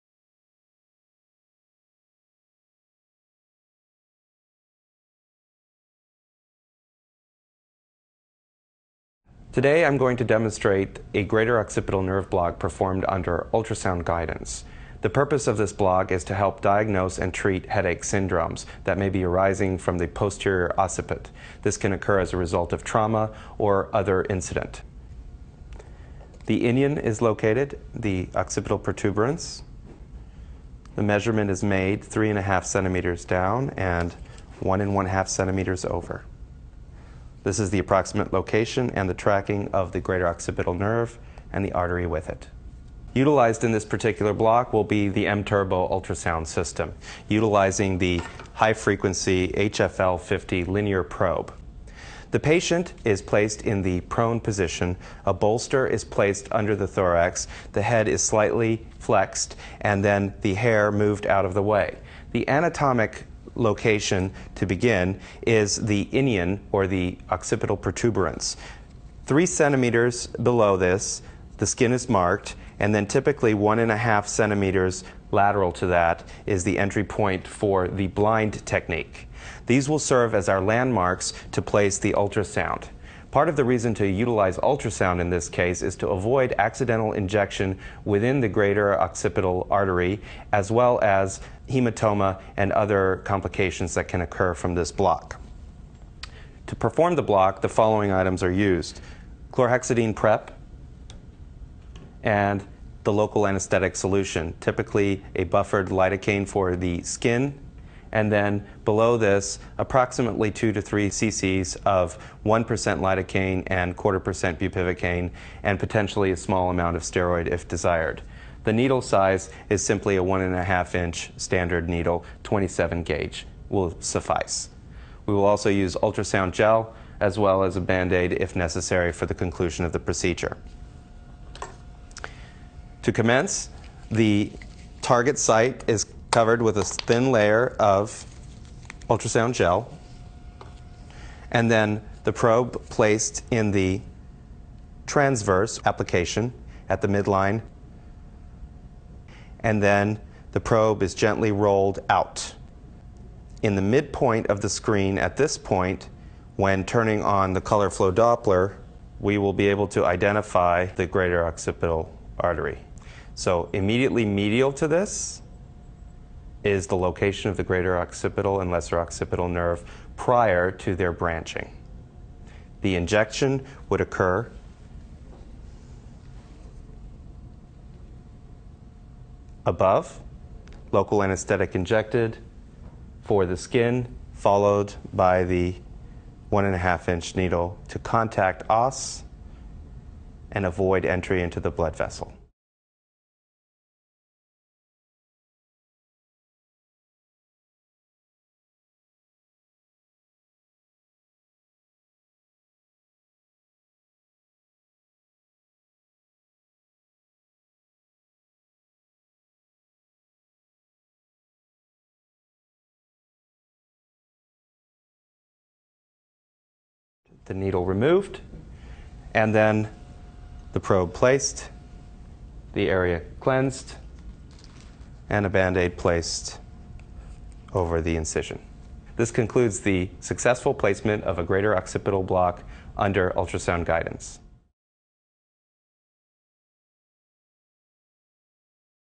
How to: Ultrasound Guided Greater Occipital Nerve Block
for more videos like this.

Dr. Michael Verdolin discusses ultrasound guidance of greater occipital nerve block. He demonstrates How to: measure the location of the greater occipital nerve followed by discussion of the transducer placement, patient position, and how color flow Doppler assists the clinician in identifying the occipital artery and occipital nerve and in improving injection accuracy.